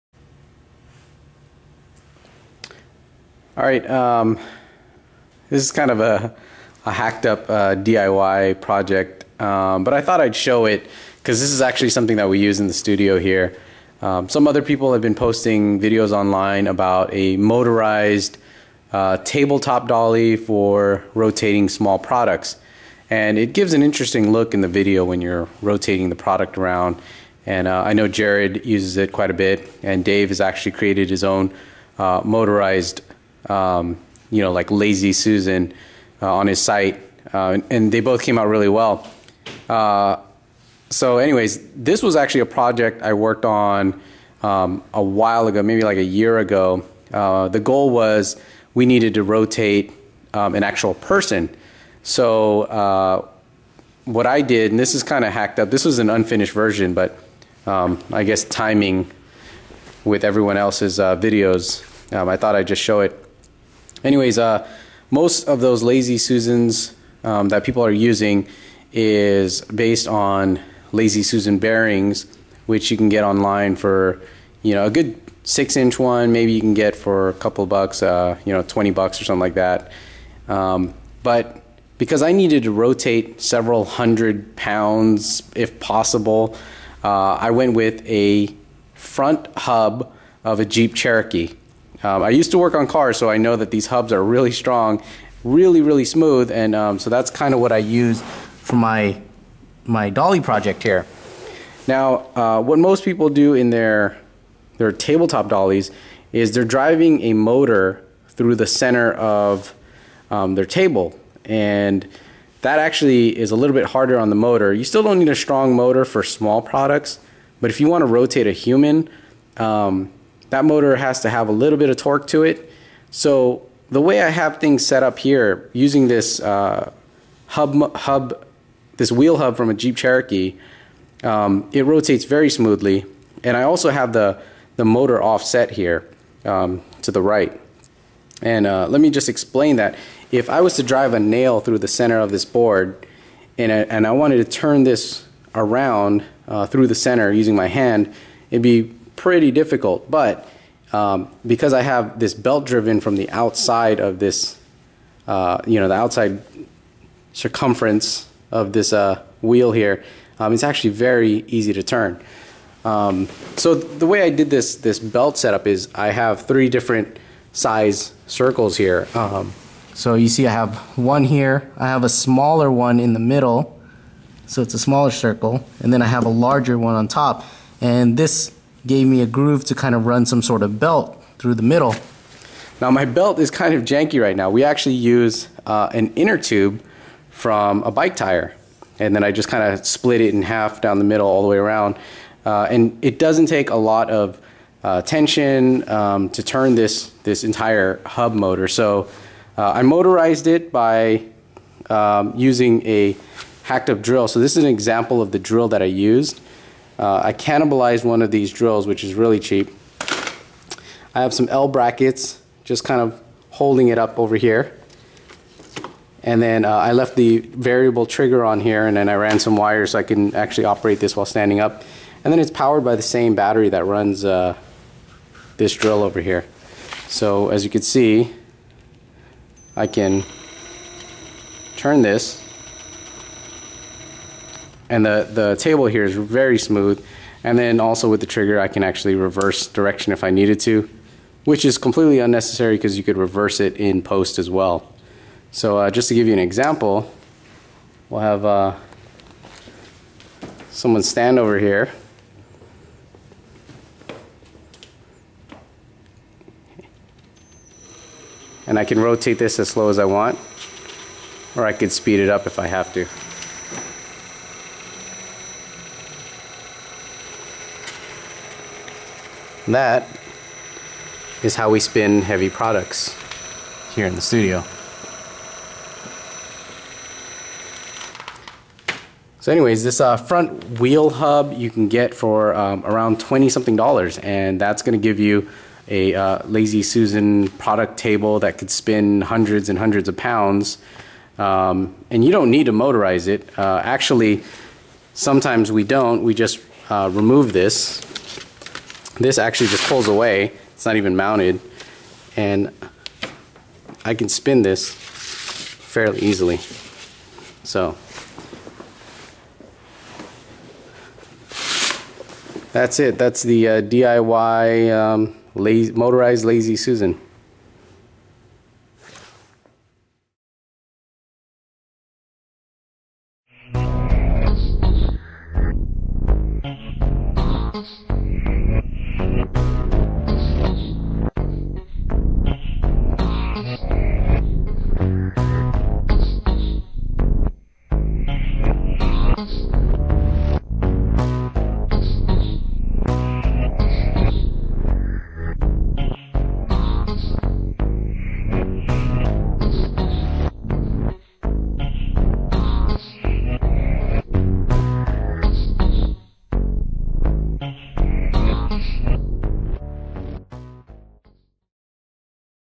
DIY Motorized Lazy Susan
A project I worked on a while ago when I was requested to create a table for spinning a person around with a green screen background. Sort of a rush job and quite embarrassing, but I thought I'd share the concept since if used properly it can add value to your production.

Using a front wheel hub which can be purchased brand new for just over $20 dollars, you can create a table that can easily spin several hundred pounds. I haven't seen anyone take this approach before. Since very little force is needed to move the hub, you can use string to pull it manually. Or it can be motorized with small motors, and can even be pulled with a simple Bicycle inner tube. No matter how much weight you put on there, it moves like butter...